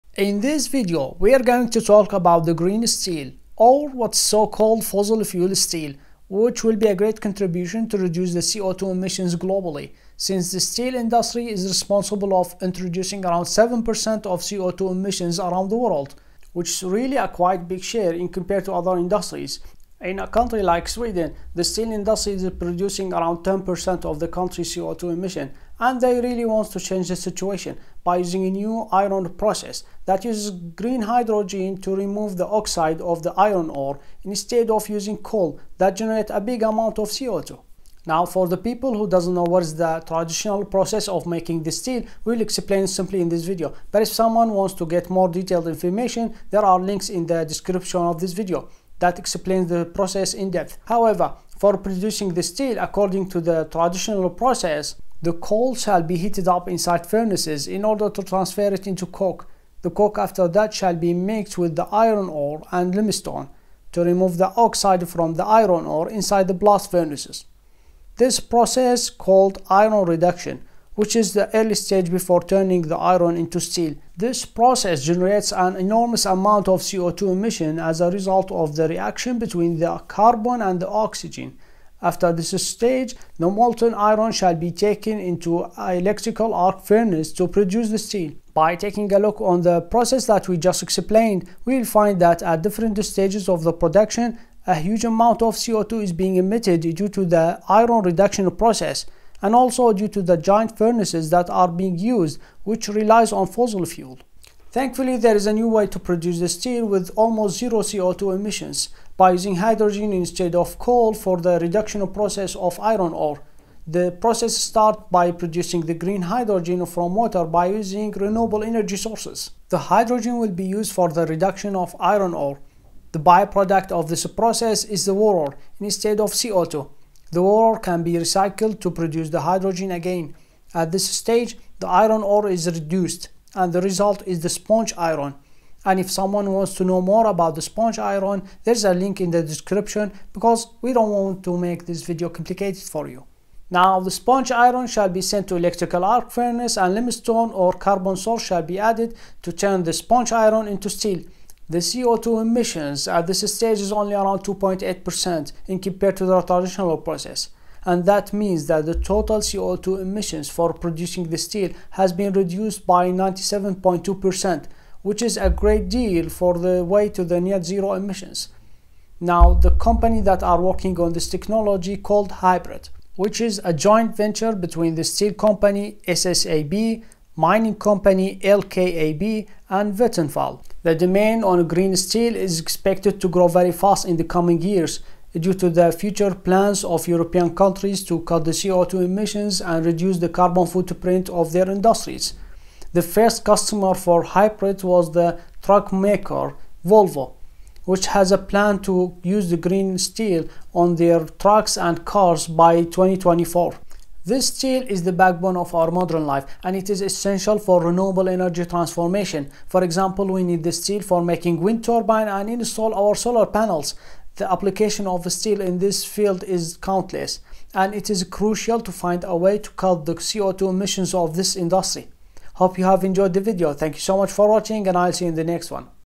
Can the Steel be sustainable ?
The Steel Industry is responsible of producing around 10% of the Co2 emissions around the world , Which is a real disaster.
The new Iron process will cut the emissions of Steel Industry by 97.2% which is very promising solution.
For producing the Steel according to the new method, The hydrogen will used instead of coal for the reduction process of Iron ore, Also the whole process shall use renewable energy sources instead of fossil fuel.

Sources :
1- Iron Process